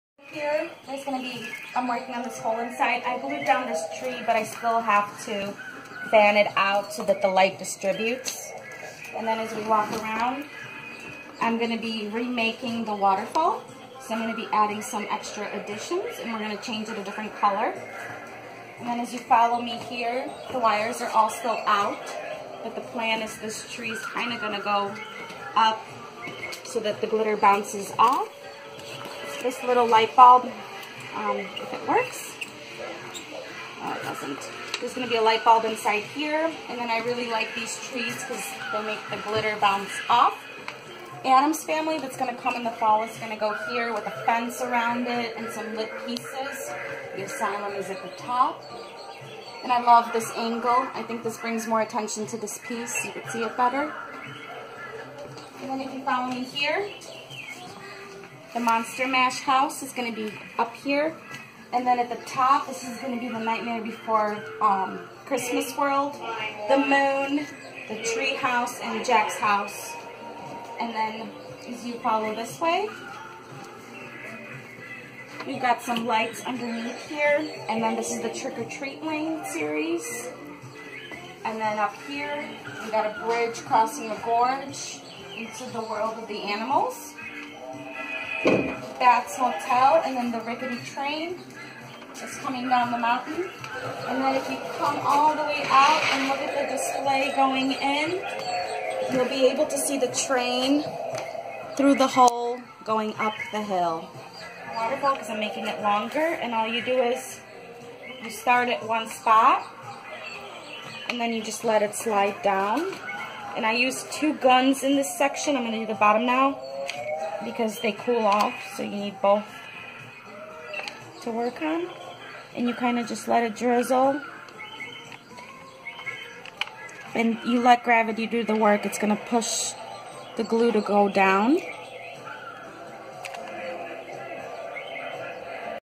Work in Progress: Department 56 Halloween Village Display
Join Irena Tomova of Department 56 as she walks us through her big plans for the renovation The Little Traveler's Halloween Village.